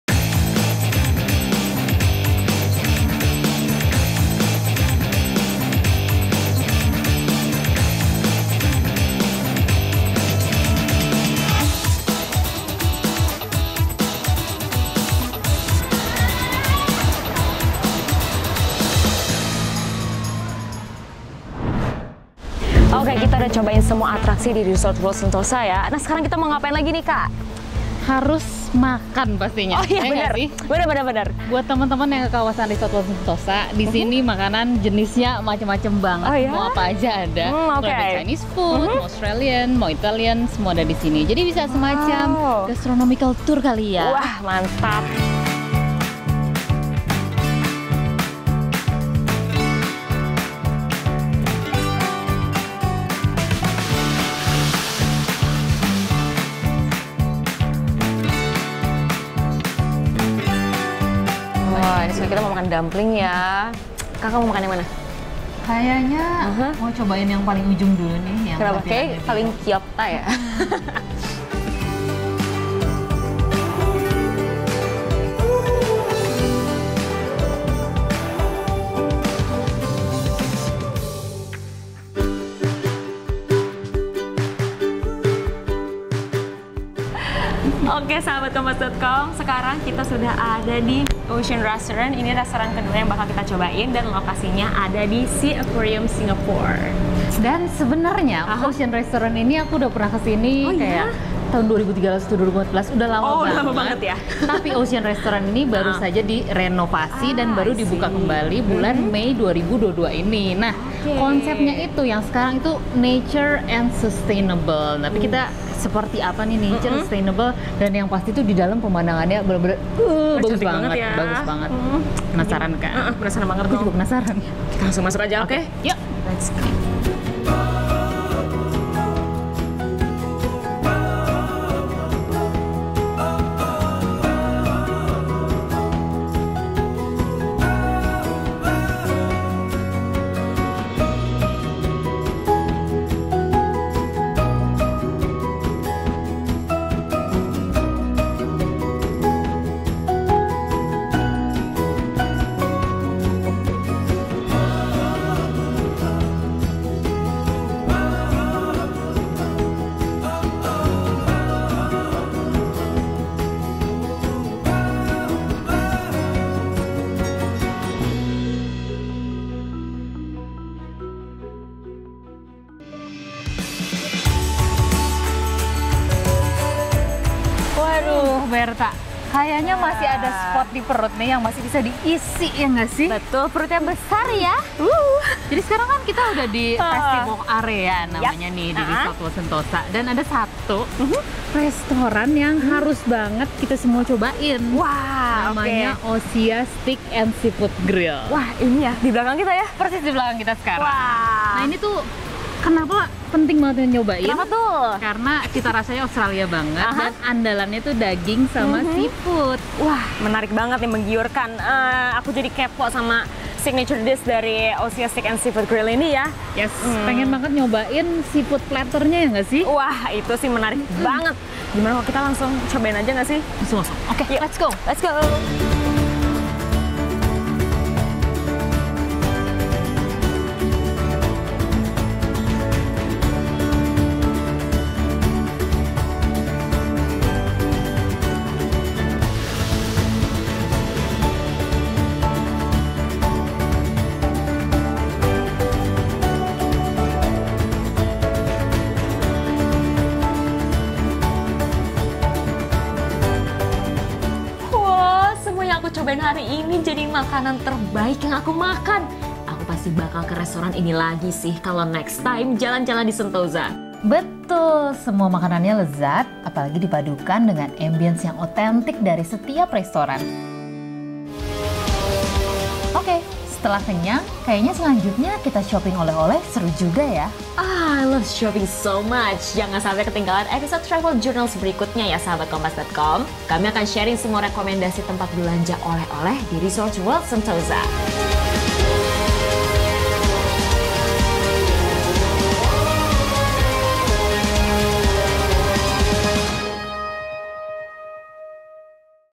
Episode 2 - Travel Journals : Kulineran di Resorts World Sentosa, Makan di dalam Akuarium!
Selain menyajikan berbagai atraksi seru, Resorts World Sentosa juga menyajikan bermacam-macam kuliner enak nan lezat yang berasal dari berbagai negara.

Dan uniknya lagi, ada salah satu tempat makan yang lokasinya berada di dalam akuarium. Keren banget 'kan, Sahabat Kompas.com?
Penasaran dalamnya seperti apa? Yuk, Sahabat Kompas.com saksikan video berikut ini!